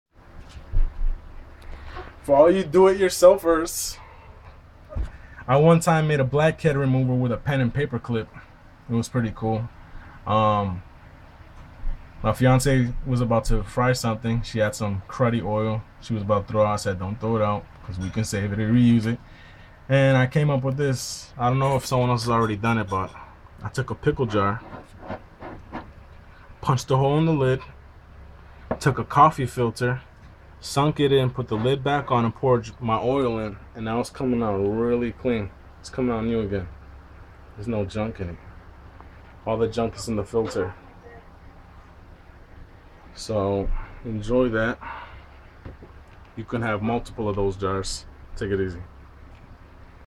DIY cooking oil filter
i Didnt want to waste the cooking oil so I did this. =)